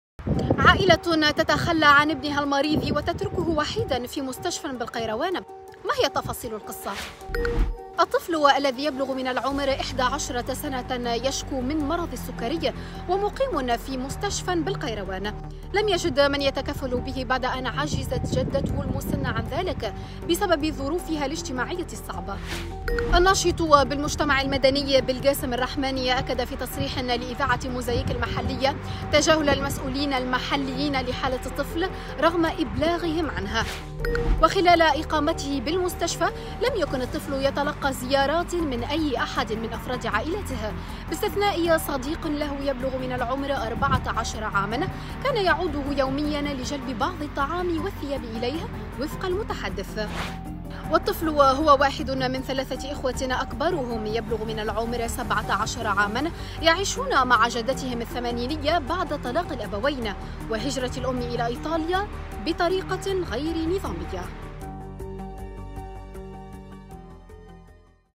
عائلة تتخلى عن ابنها المريض وتتركه وحيدا في مستشفى بالقيروان
عائلة تتخلى عن ابنها المريض وتتركه وحيدا في مستشفى بالقيروان.. تعرف على تفاصيل القصة